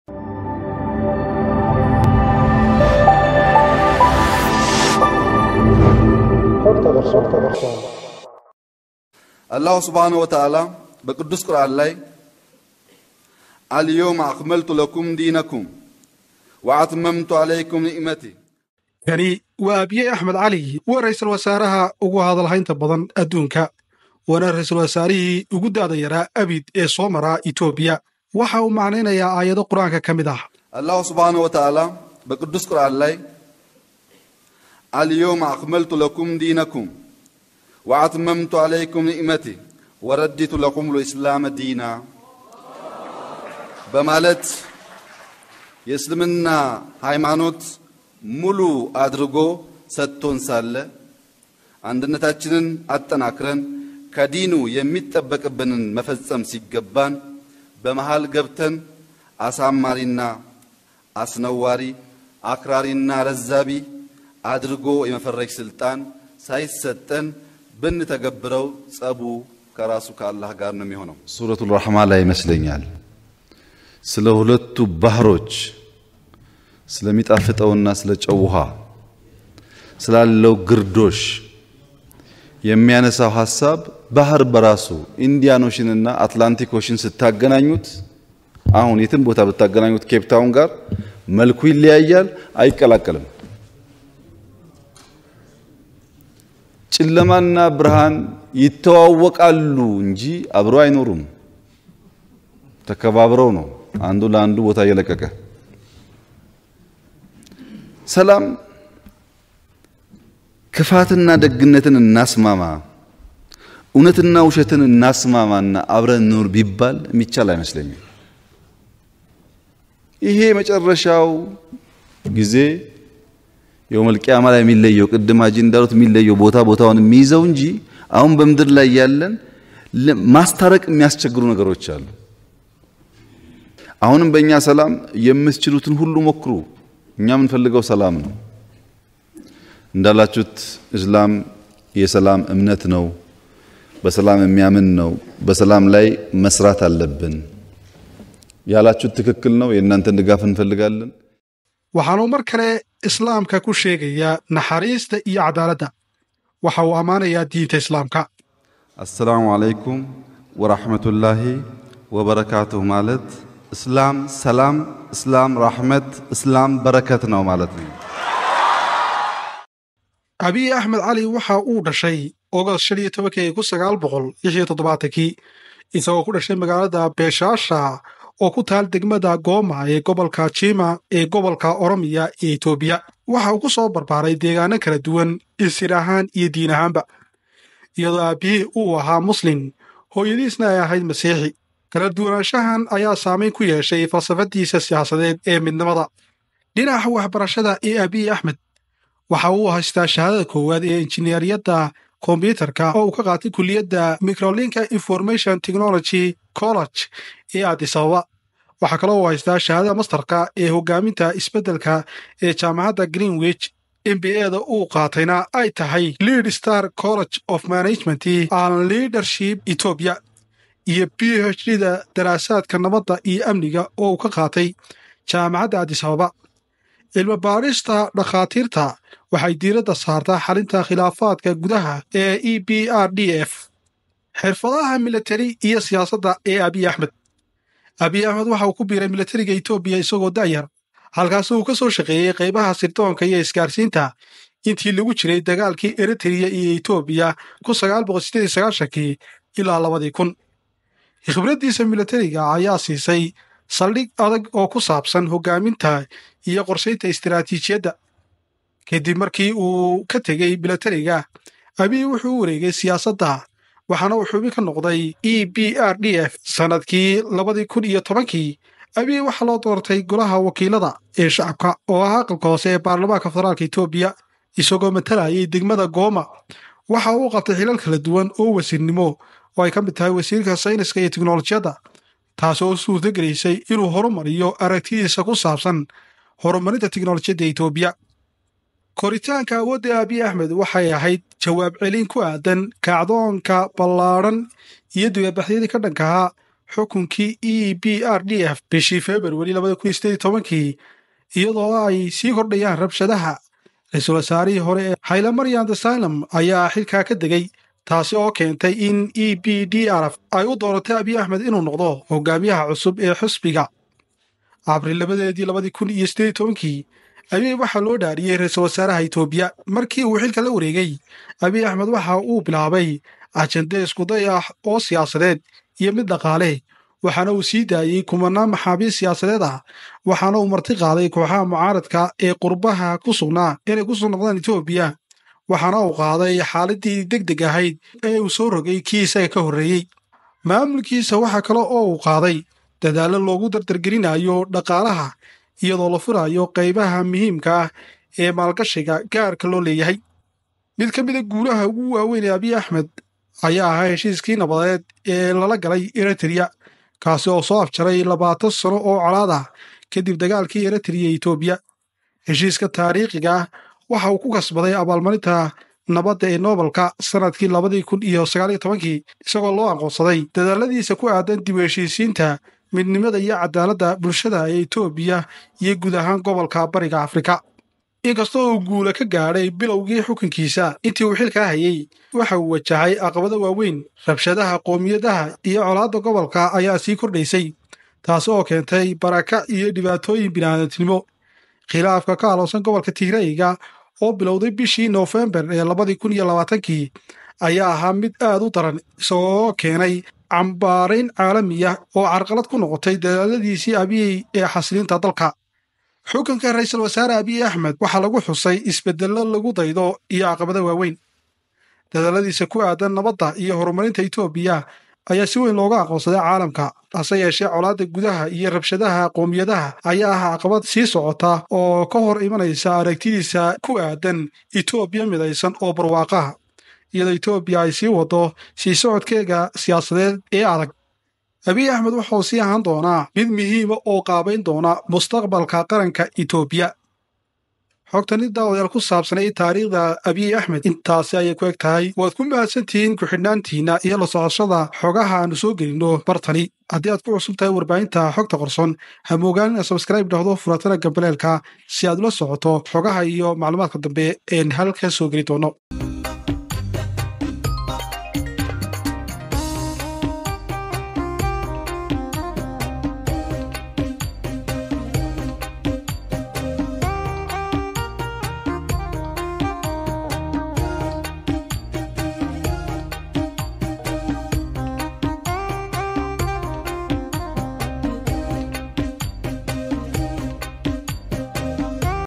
Muslim Miyaa Abiy Ahmed & Tariikhdisa.
history abiy ahmed
abiy ahmed and muslims
Kani Abiy Ahmed Ali  waa ra’iisal wasaarahi  ugu hadal haynta badan aduunka ahna raysal wasaaraiihi ugu da’da yara abiid ee soo mara  itoobiya (Ethiopia).
Waxa uu macnanayna ayaadao quraanka ka ka mid ah, waxana uu ku salinaya nabiga…. Waxa markale islamka ku sheegaya naxariista iyo cadaala…. Waxuu amaanaya diinta islamka.
Abiy Ahmed Ali waxa uu dhashay August 15, 1976, magaalada Beshasha, oo ku taalla degmada Gomma ee gobolka Jimma, ee gobolka Oromia ee Itoobiya. Waxa uu ku soo barbaaray deegaan kala duwan oo isir iyo diinba ah, iyadoo aabbihiis uu ahaa Muslim hooyadiisna ay ahayd Masiixi. Kala duwanaanshahan ayaa saameyn ku yeeshay falsafaddiisa siyaasadeed ee midnimada.